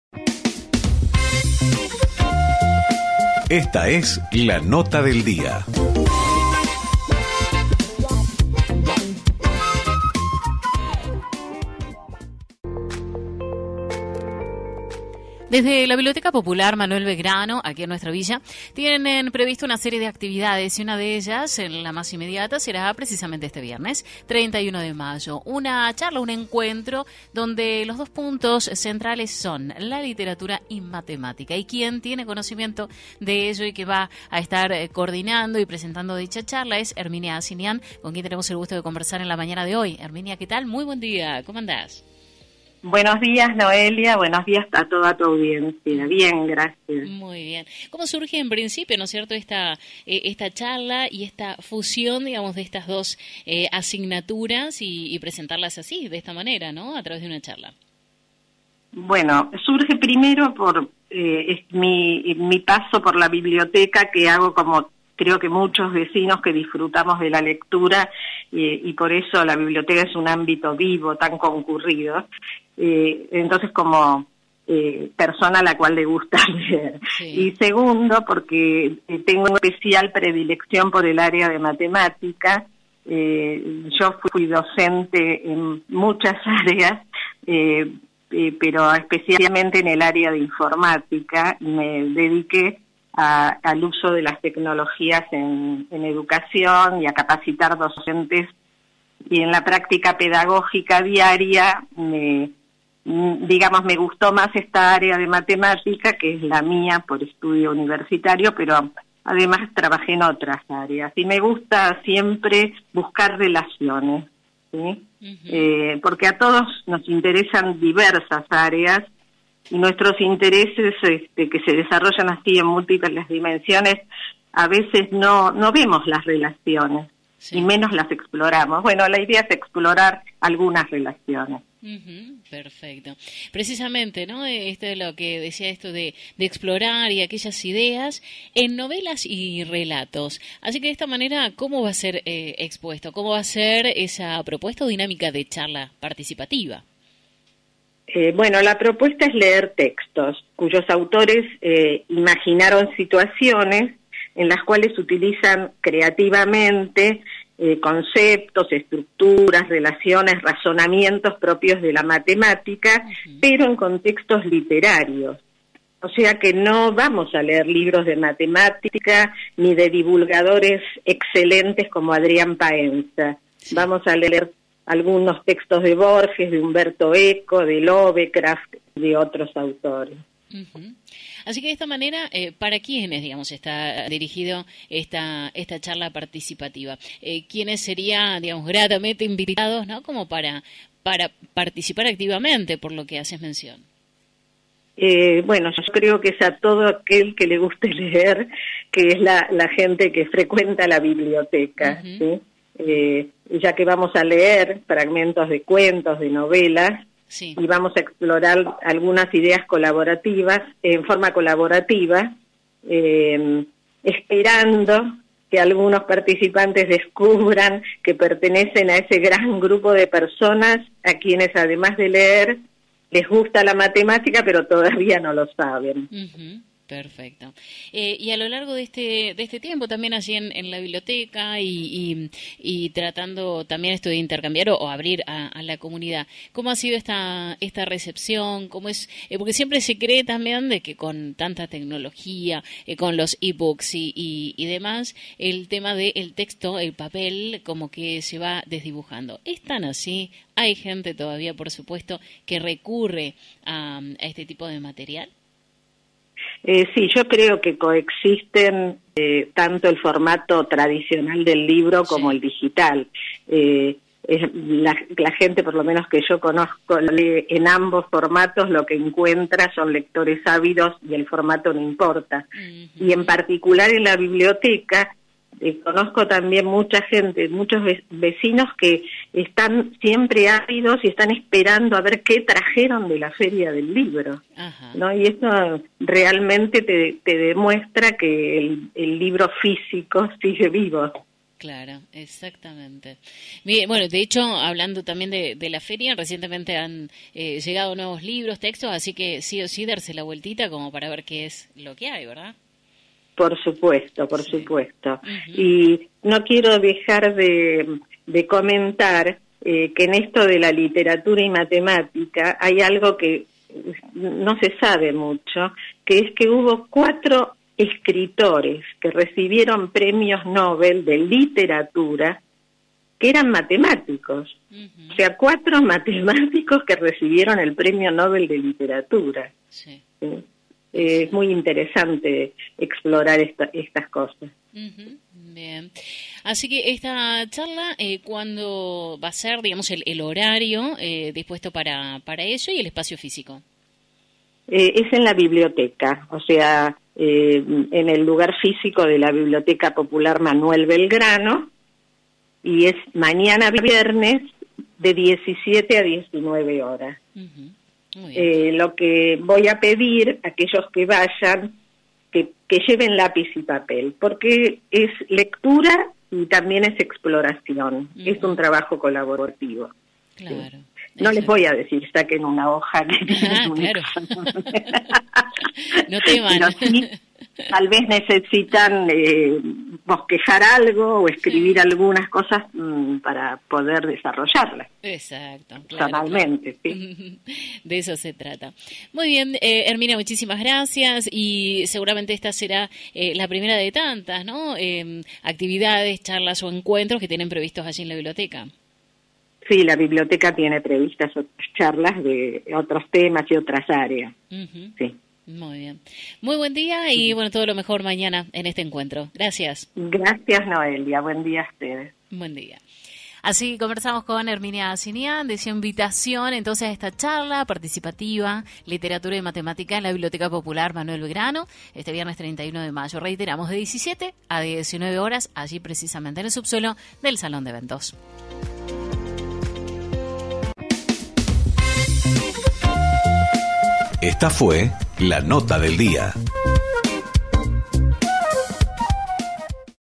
101.9FM Herminia Azinian, quien invita a la charla sobre literatura y matemática en la Biblioteca
En el aire de la 101.9FM Herminia Azinian, quien invita a la charla sobre literatura y matemática en la Biblioteca Popular Manuel Belgrano, este viernes 31 de mayo.

30.05.24